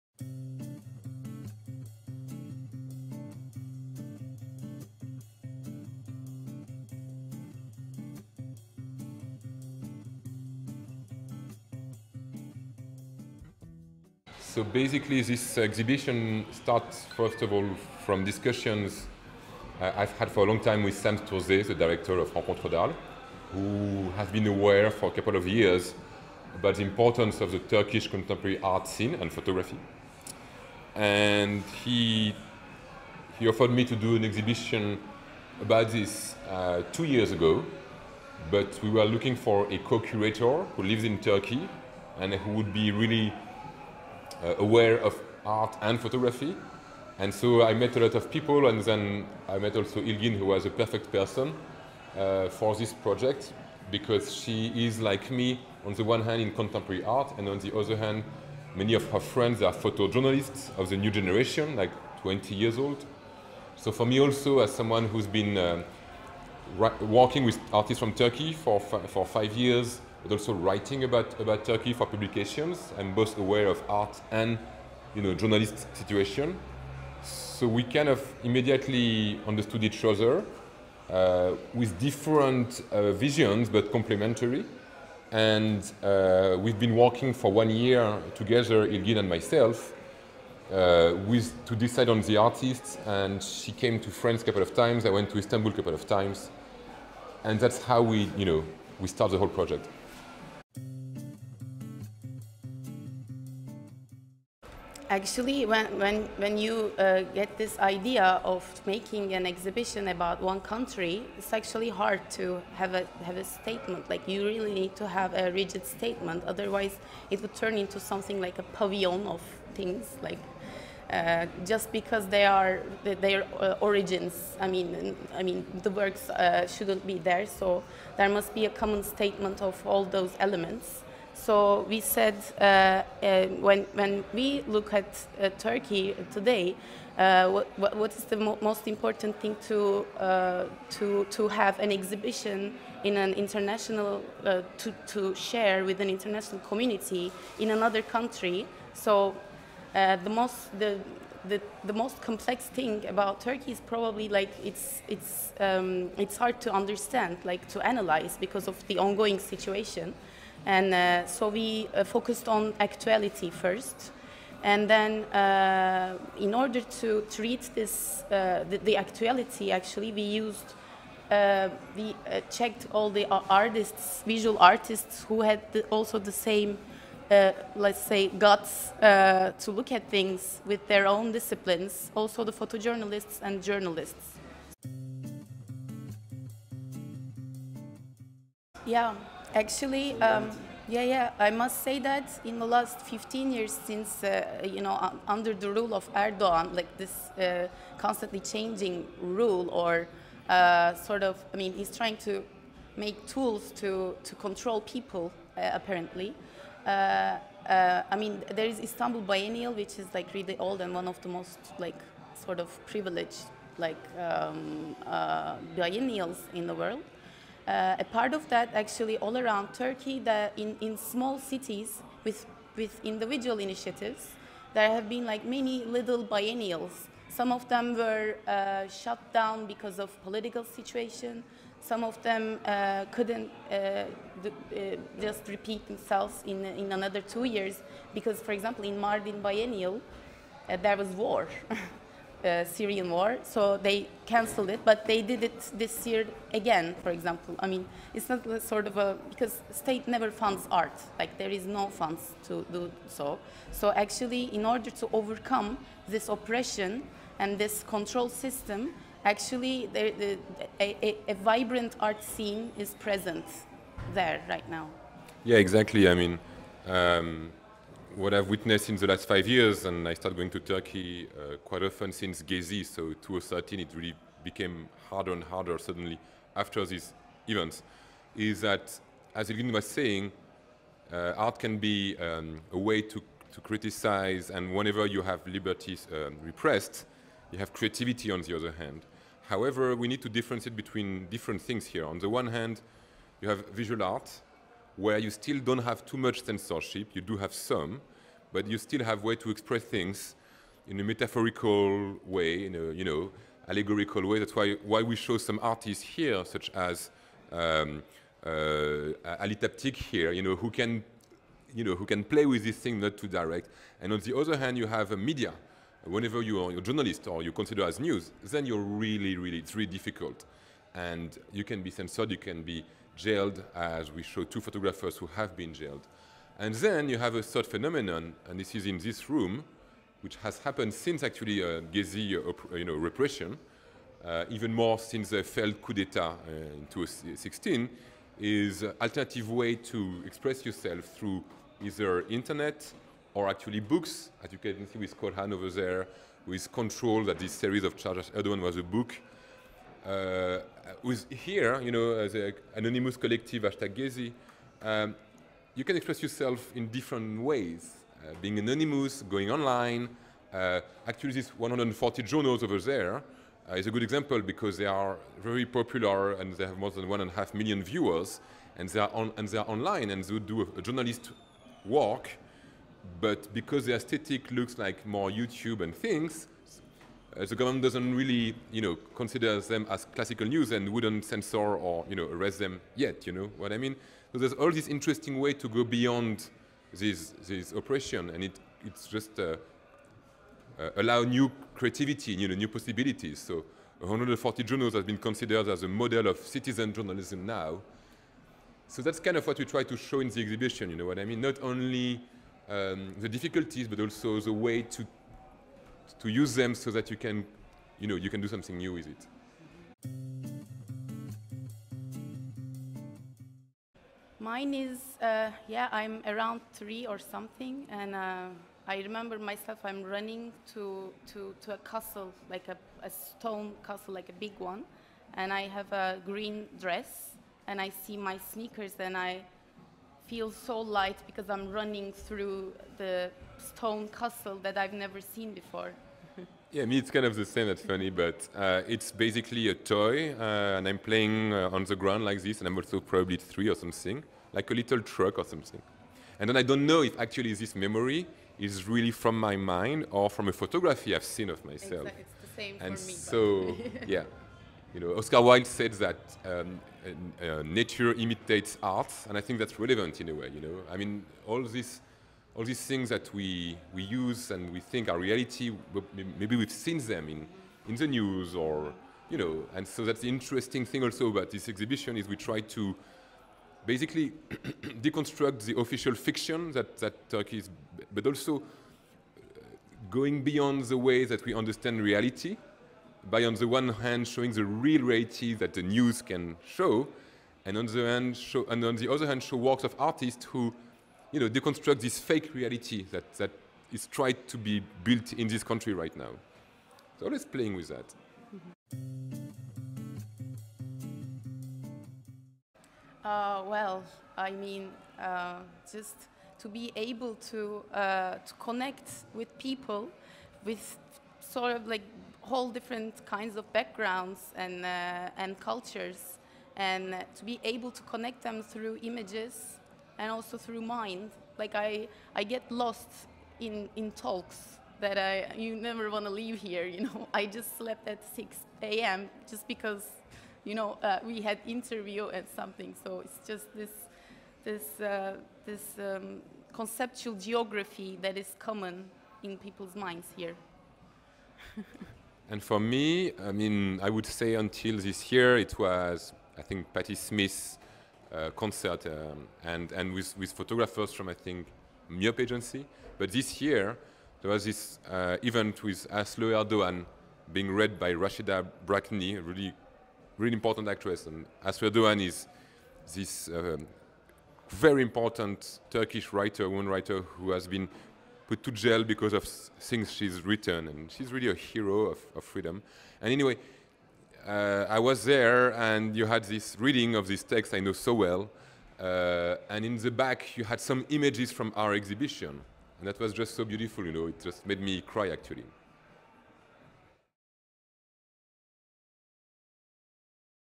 Interview Ilgın Deniz Akseloğlu & Yann Perreau
Interview de Ilgın Deniz Akseloğlu et Yann Perreau, commissaires de l’exposition Une colonne de fumée, Regards sur la scène contemporaine turque.

Interview of  Ilgın Deniz Akseloğlu et Yann Perreau, curators the exhibition A pillar of smoke, A look at Turkey’s contemporary scene.